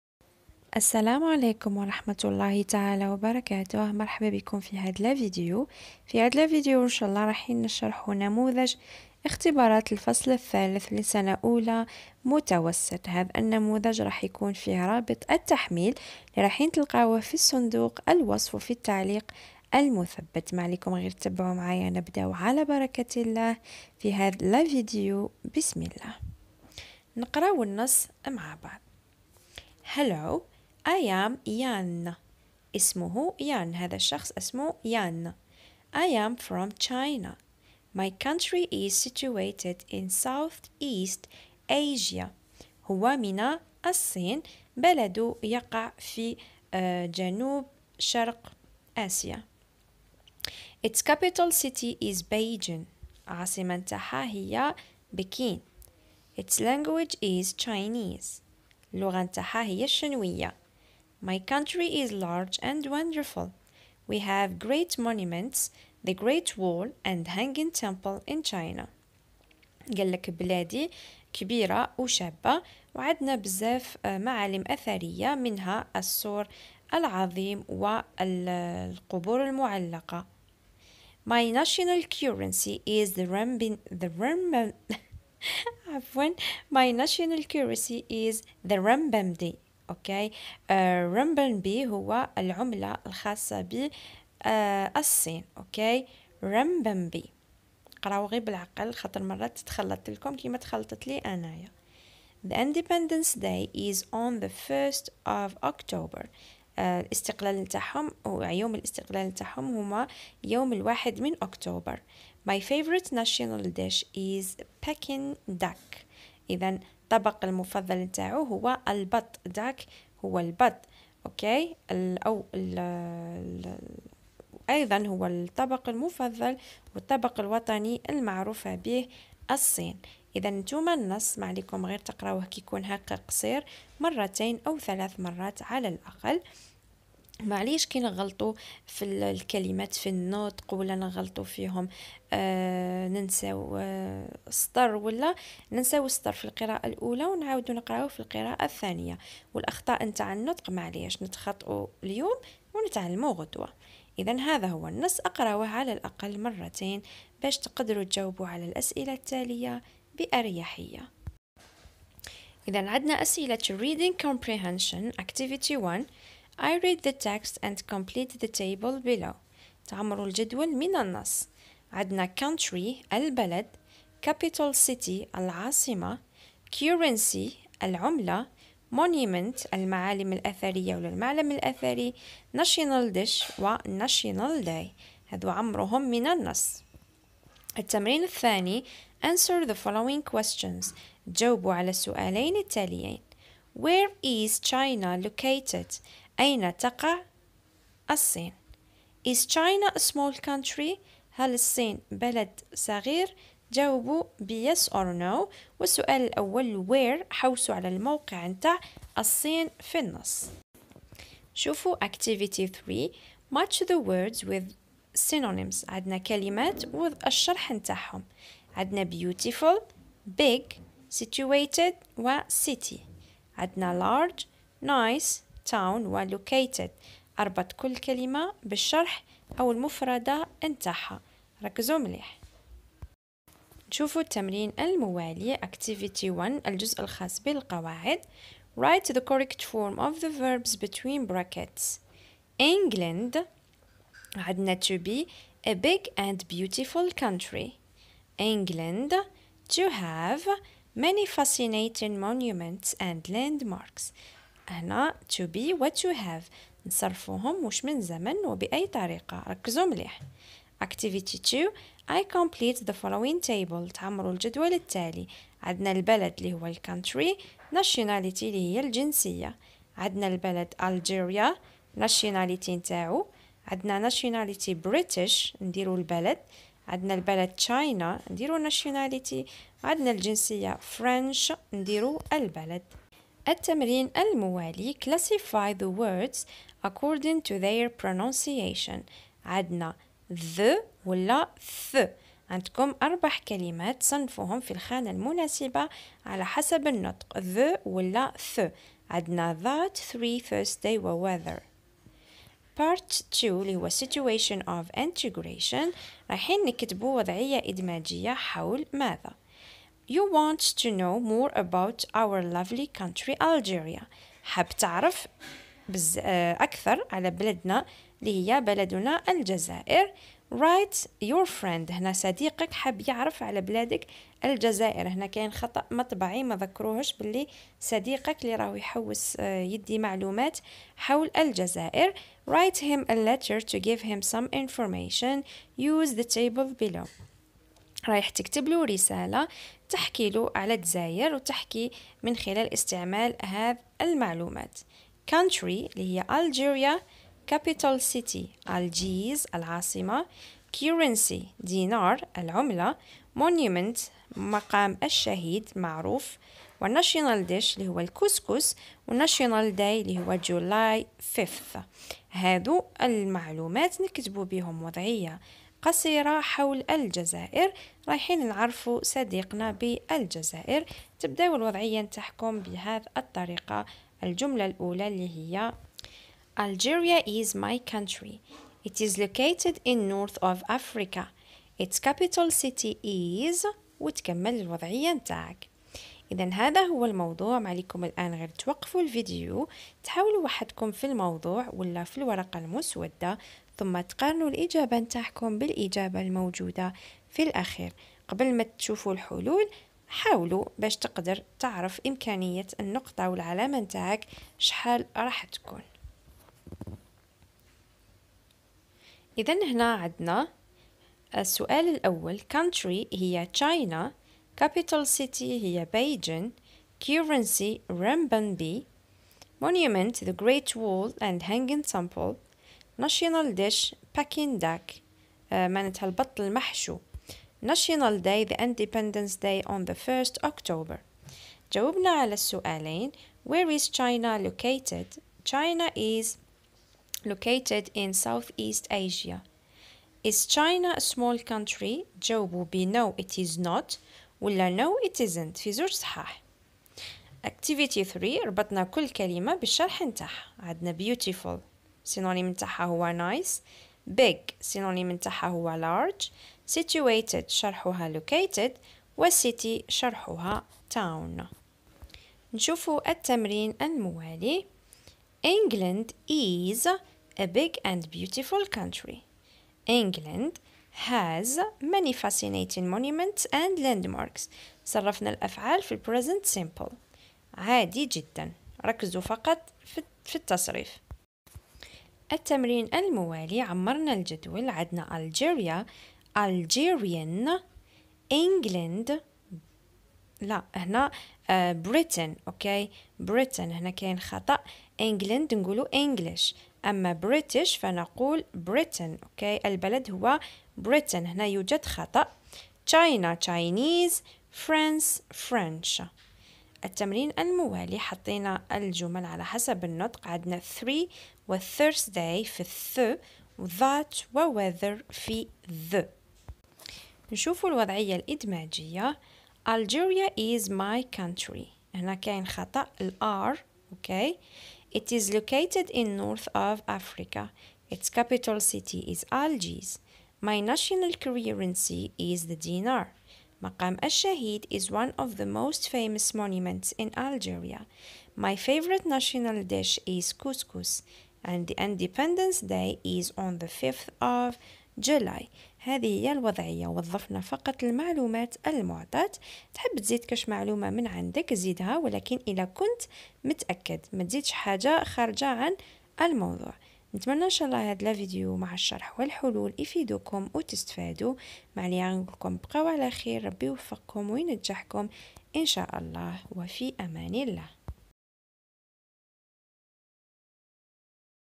اختبارات الفصل الثالث محلولة لسنة اولى متوسط لغة انجليزية مع الشرح ورابط التحميل حول العملة والبلد
اختبارات الفصل الثالث محلولة لسنة اولى متوسط لغة انجليزية
رابط التحميل